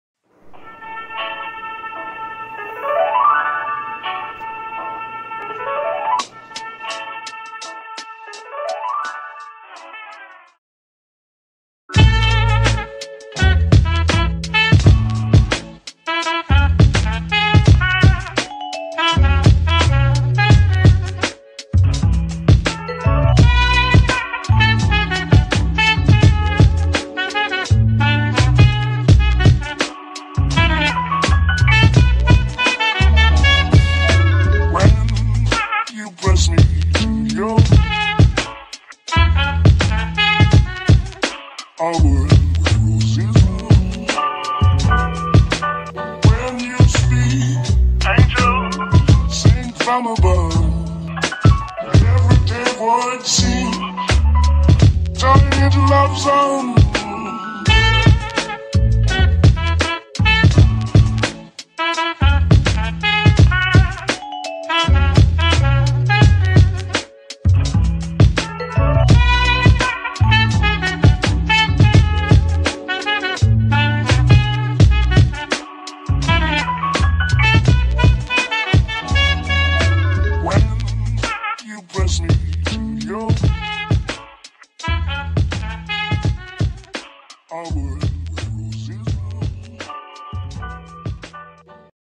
Painting my desks in to white | My old desk look dope in white 🤩
This is how I've painted my desks and they look so delicious 😍